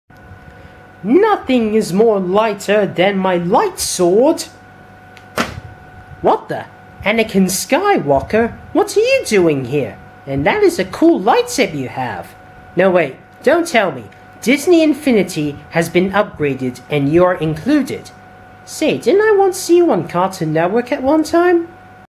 BRR's Fun with Figures: Disney Inifinity and Skylanders - The lighter sword
Knight Light being of the Light element has a light sword. He has stumbled across another person with a Light sword.